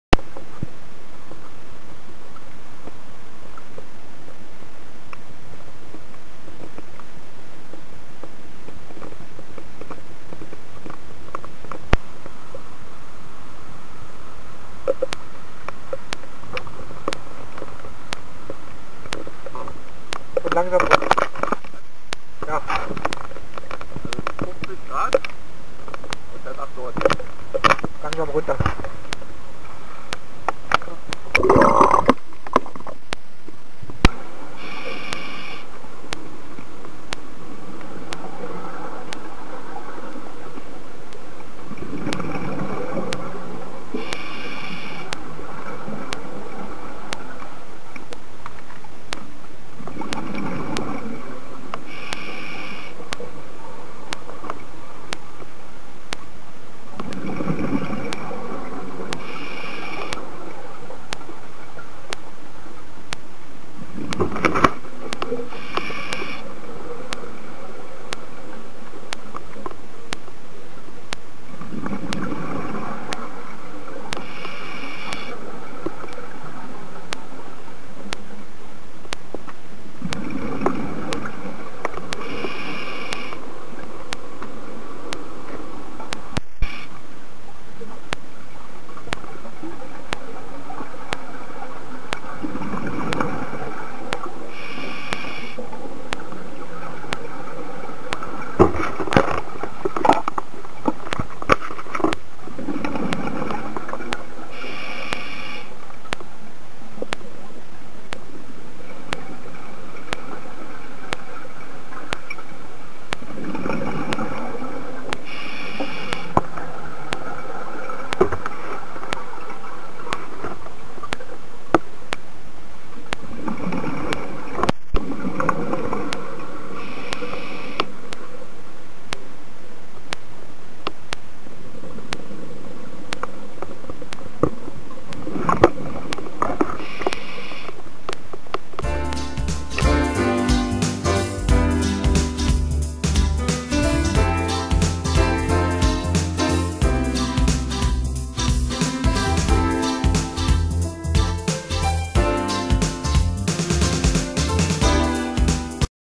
Werbellinsee - Am Dornbusch
Taucher@Net - Der Kaffenkahn am Dornbusch (No 1) - gefilmt von Maik